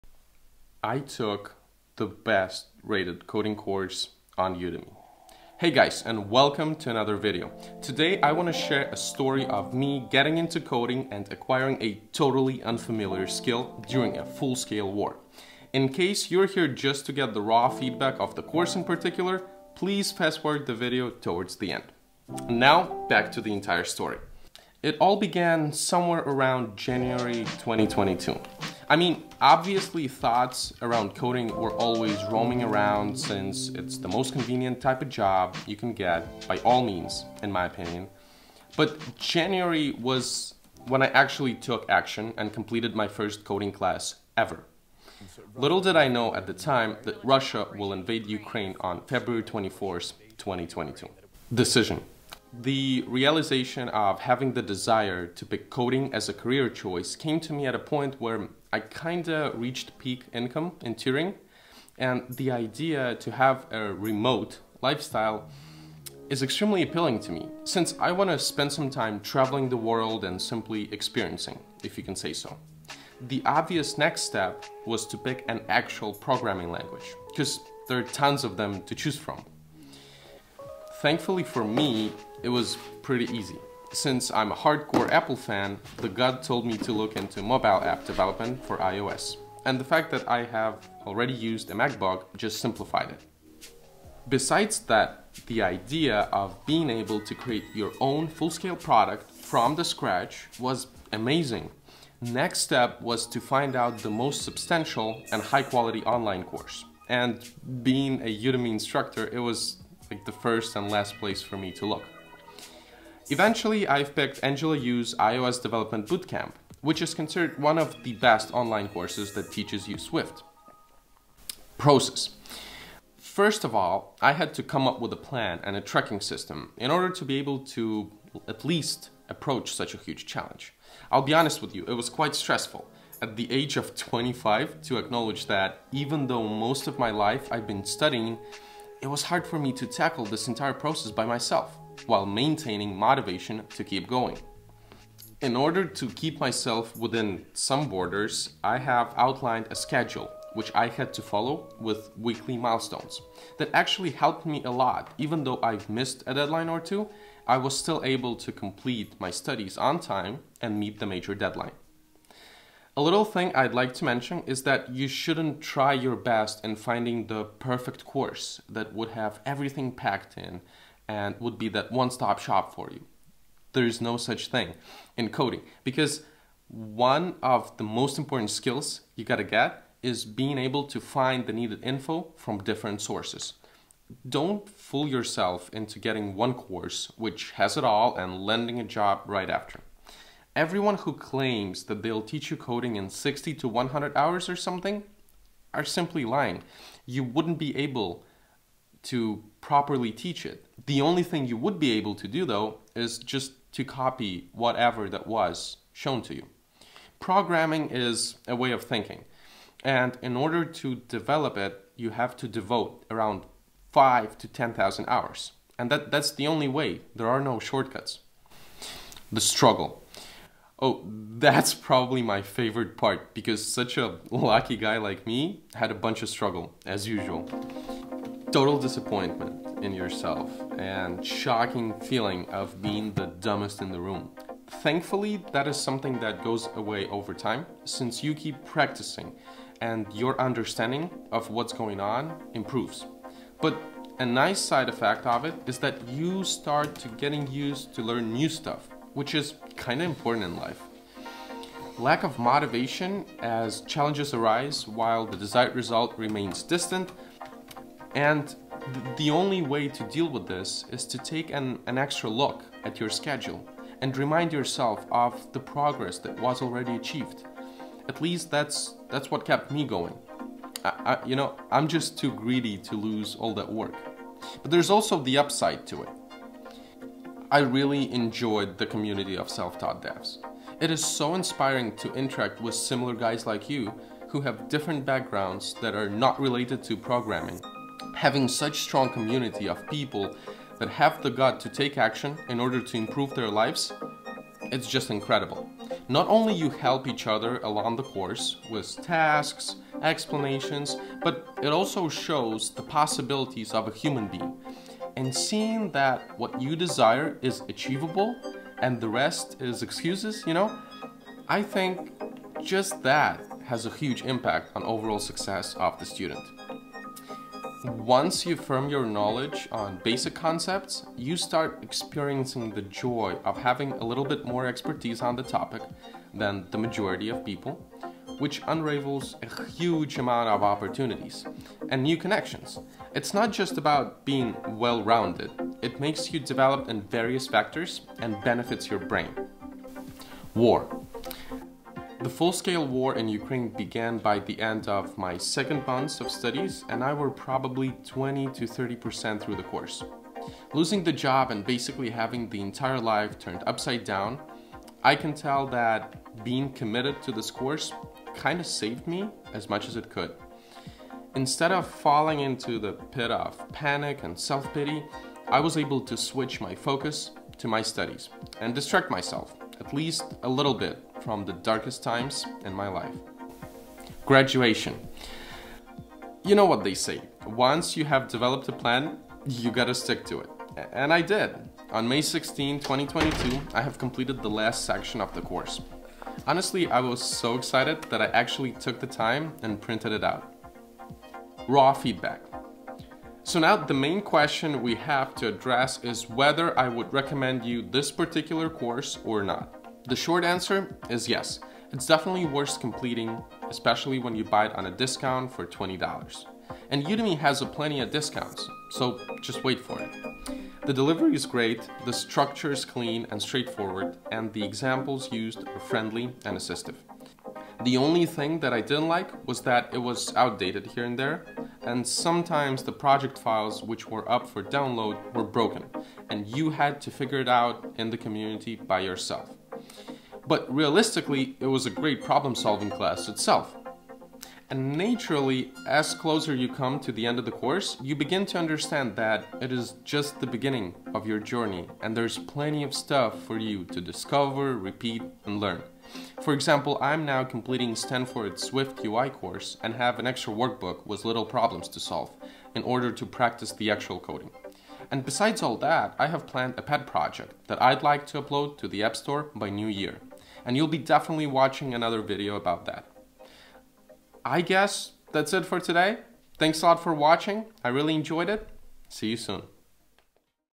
I took the best coding course on Udemy
I took Angela Yu's Swift bootcamp in order to learn how to develop iOS apps.